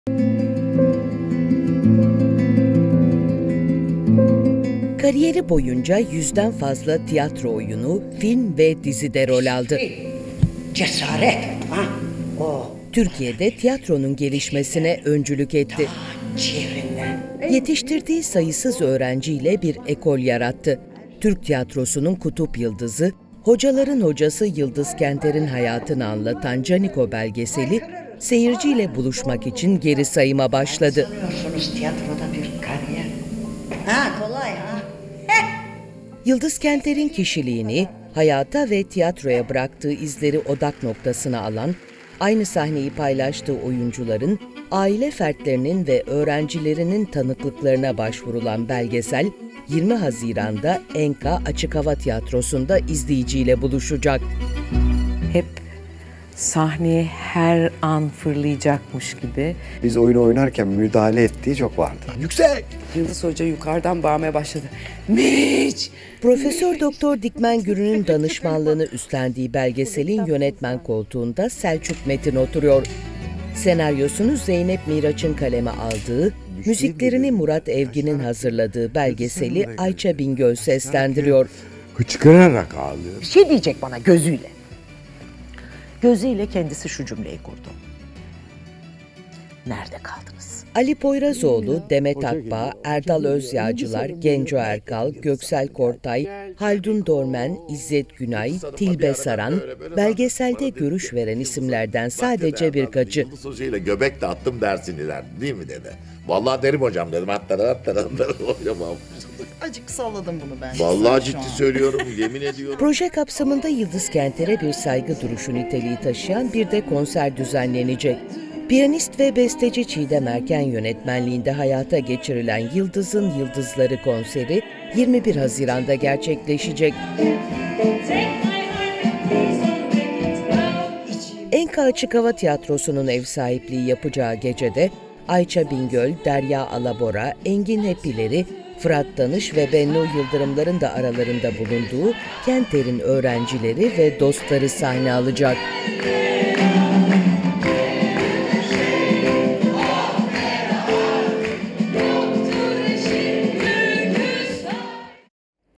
Yıldız Kenter'in hayatını anlatan "Caniko" belgeseli seyirci ile buluşmaya hazırlanıyor
"Türk tiyatrosunun kutup yıldızı", hocaların hocası Yıldız Kenter'in hayatını anlatan "Caniko" belgeseli seyirci ile buluşmak için geri sayıma başladı.

Yıldız Kenter'in kişiliğini, hayata ve tiyatroya bıraktığı izleri odak noktasına alan, aynı sahneyi paylaştığı oyuncuların, aile fertlerinin ve öğrencilerinin tanıklıklarına başvurulan belgesel, 20 Haziran'da ENKA Açıkhava Tiyatrosu'nda izleyiciyle buluşacak.